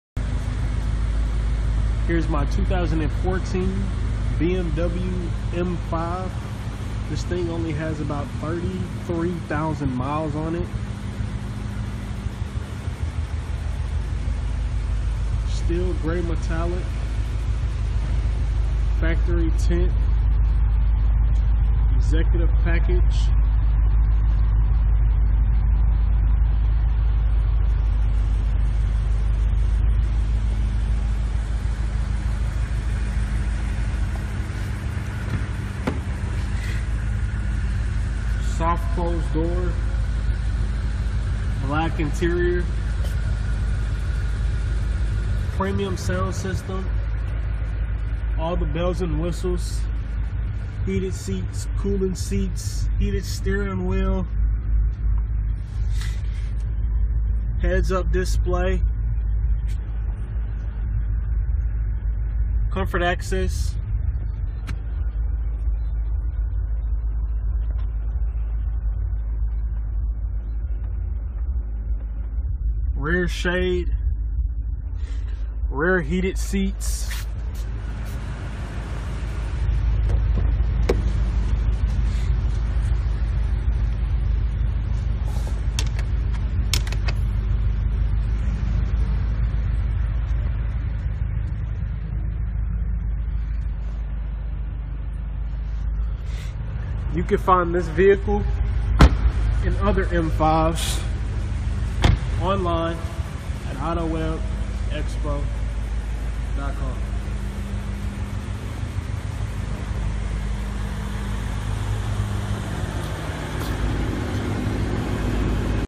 2014 BMW M5
Steel grey metallic M5 under 50k miles, hard loaded & at a discounted price right now.